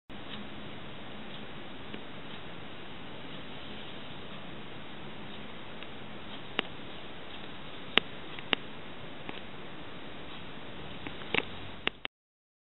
Homemade Fisheye Lens
Just a very short test video with my new homemade fisheye lens.